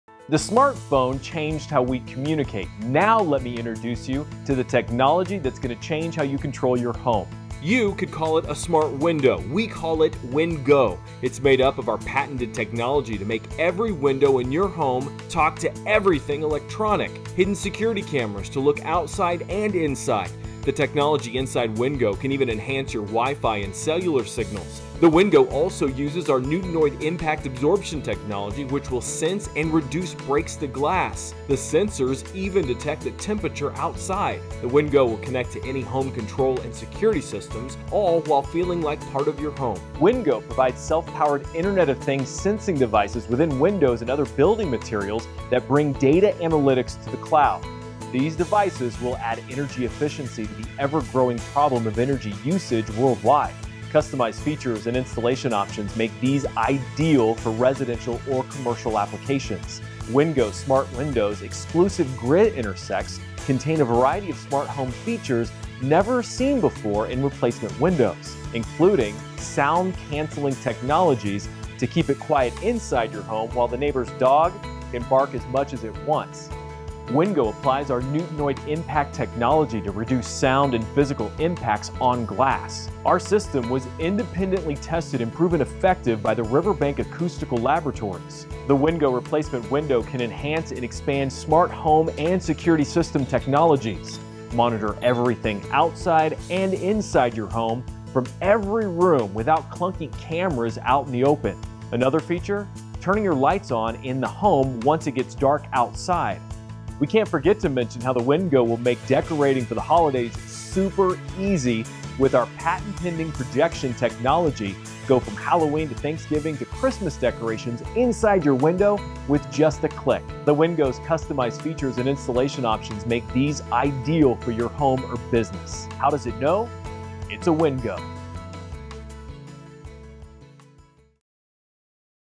WINDGO Smart Windows
WINDGO Smart Windows are a new innovation in windows for the home and business. Using patented technology to provide impact absorption, reduce sound transference and more, WINDGO Smart Windows are a smart investment for your home.